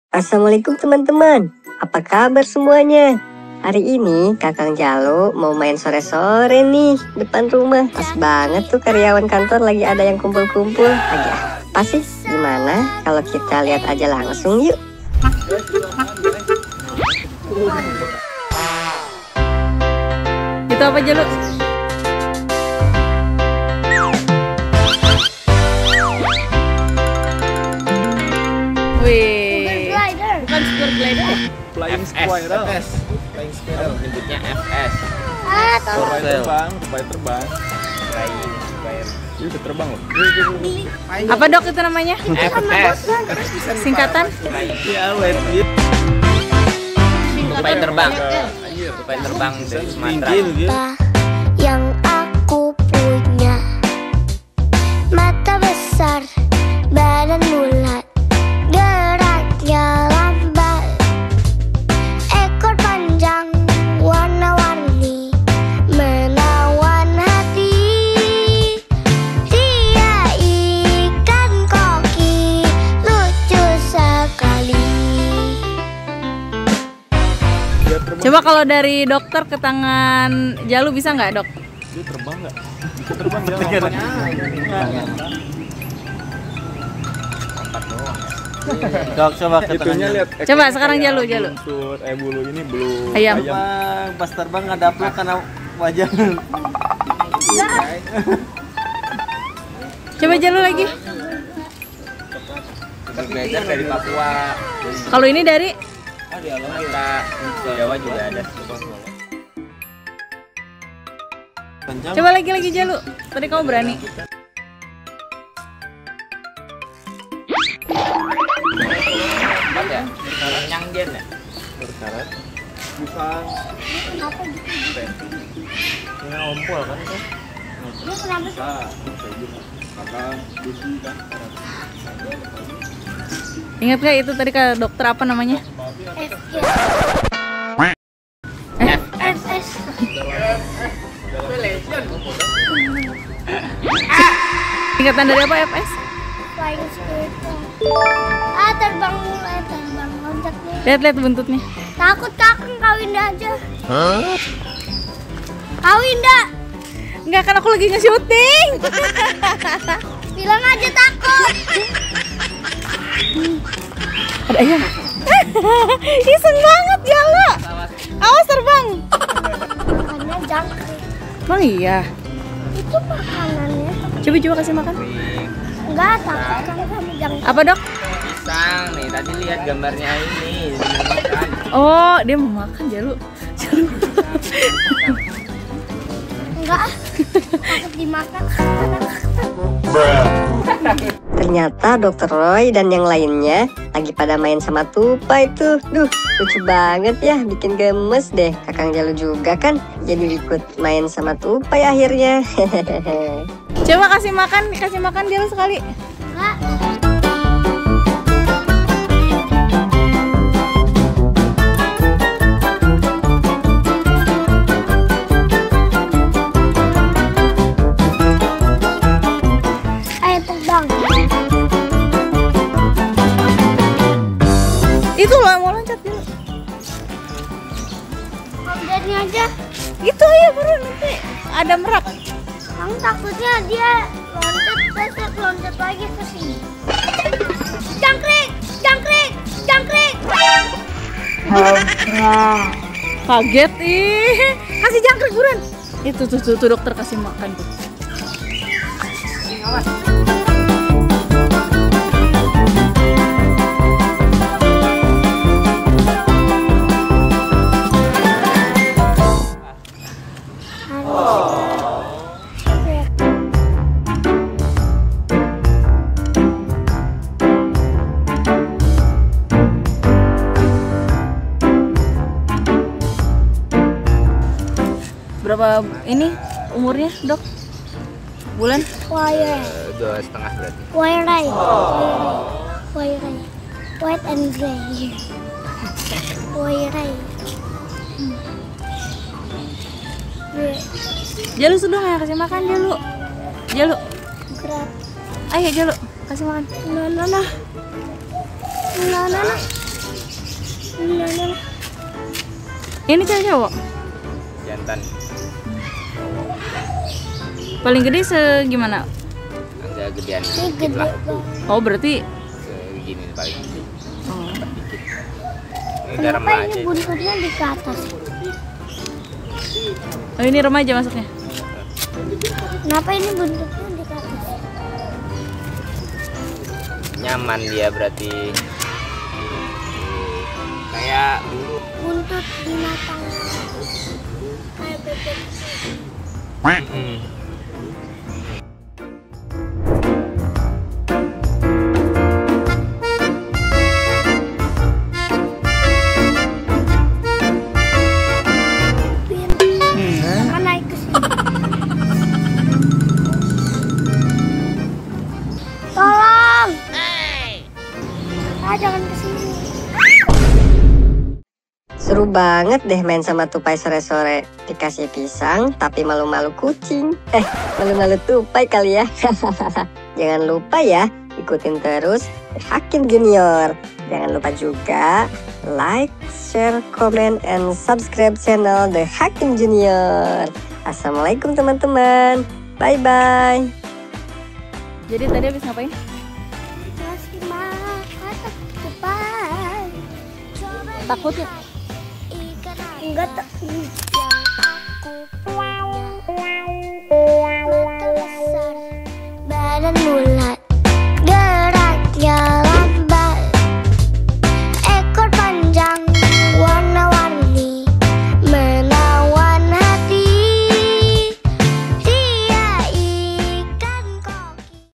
KASIH MAKAN TUPAI TERBANG.. BIKIN GEMES BANGET..!! TAPI TAKUT 😅
Kakang Djalu main sore-sore keluar nih..
Liat hewan gemes lagi dikerumunin tim Keeper dehakims..